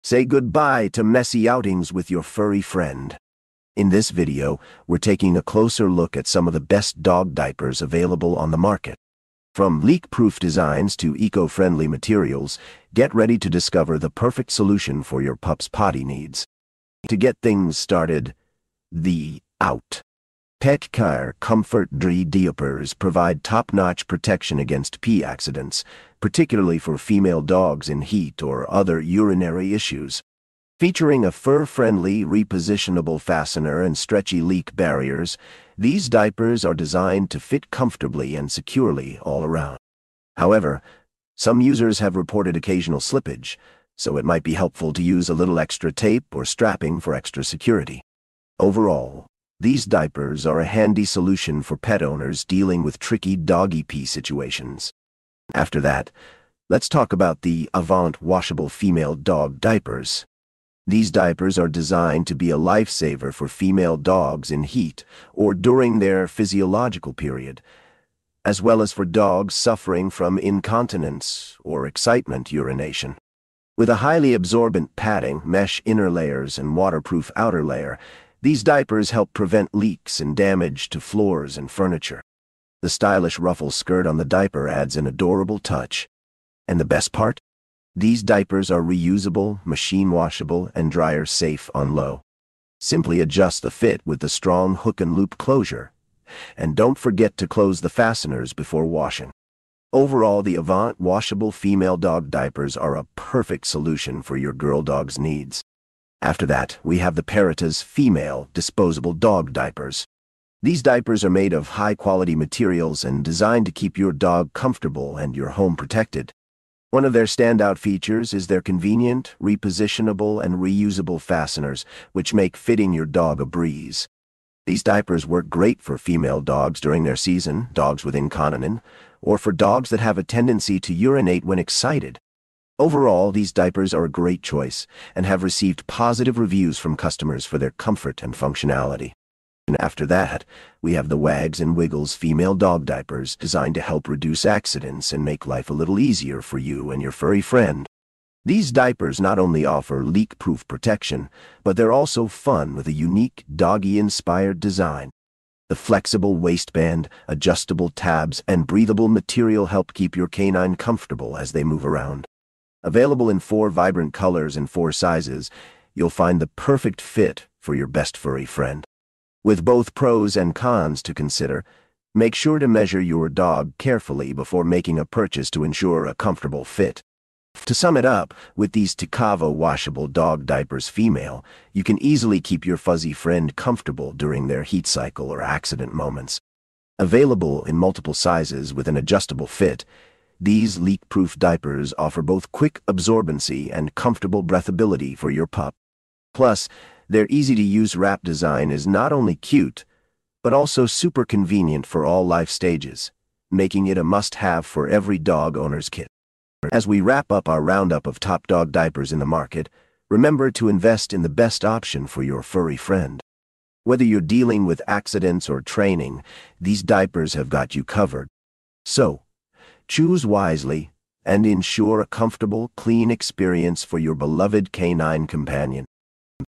The Best Dog Diapers | SERP Reviews
Dog Diapers: Our Top Picks for The Best Dog Diapers

00:00 Introduction to Dog Diapers
00:16 OUT! Comfort
00:53 Washable Dog Diapers for Female Dogs
01:43 Comfortable Disposable Dog Diapers for All Stages
02:52 Premium Reusable Dog Diapers for Female Dogs
03:21 Conclusion and Wrap-up

Say goodbye to messy outings with your furry friend! In this video, we're taking a closer look at some of the best dog diapers available on the market. From leak-proof designs to eco-friendly materials, get ready to discover the perfect solution for your pup's potty needs.

To get things started, the OUT! Petcare Comfort-Dri Diapers provide top-notch protection against pee accidents, particularly for female dogs in heat or other urinary issues. Featuring a fur-friendly, repositionable fastener and stretchy leak barriers, these diapers are designed to fit comfortably and securely all around. However, some users have reported occasional slippage, so it might be helpful to use a little extra tape or strapping for extra security. Overall, these diapers are a handy solution for pet owners dealing with tricky doggy pee situations.

After that, let's talk about the Avont Washable Female Dog Diapers. These diapers are designed to be a lifesaver for female dogs in heat or during their physiological period, as well as for dogs suffering from incontinence or excitement urination. With a highly absorbent padding, mesh inner layers, and waterproof outer layer, these diapers help prevent leaks and damage to floors and furniture. The stylish ruffle skirt on the diaper adds an adorable touch. And the best part? These diapers are reusable, machine-washable, and dryer safe on low. Simply adjust the fit with the strong hook-and-loop closure, and don't forget to close the fasteners before washing. Overall, the Avont Washable Female Dog Diapers are a perfect solution for your girl dog's needs.

After that, we have the Peritas Female Disposable Dog Diapers. These diapers are made of high-quality materials and designed to keep your dog comfortable and your home protected. One of their standout features is their convenient, repositionable, and reusable fasteners, which make fitting your dog a breeze. These diapers work great for female dogs during their season, dogs with incontinence, when traveling, or for dogs that have a tendency to urinate when excited. Overall, these diapers are a great choice and have received positive reviews from customers for their comfort and functionality.

After that, we have the Wags & Wiggles Female Dog Diapers, designed to help reduce accidents and make life a little easier for you and your furry friend. These diapers not only offer leak-proof protection, but they're also fun with a unique doggie-inspired design. The flexible waistband, adjustable tabs, and breathable material help keep your canine comfortable as they move around. Available in four vibrant colors and four sizes, you'll find the perfect fit for your best furry friend. With both pros and cons to consider, make sure to measure your dog carefully before making a purchase to ensure a comfortable fit.

To sum it up, with these Tsikavo Washable Dog Diapers Female, you can easily keep your fuzzy friend comfortable during their heat cycle or accident moments. Available in multiple sizes with an adjustable fit, these leak-proof diapers offer both quick absorbency and comfortable breathability for your pup. Plus, their easy-to-use wrap design is not only cute but also super convenient for all life stages, making it a must-have for every dog owner's kit.

As we wrap up our roundup of top dog diapers in the market, remember to invest in the best option for your furry friend. Whether you're dealing with accidents or training, these diapers have got you covered. So, choose wisely and ensure a comfortable, clean experience for your beloved canine companion.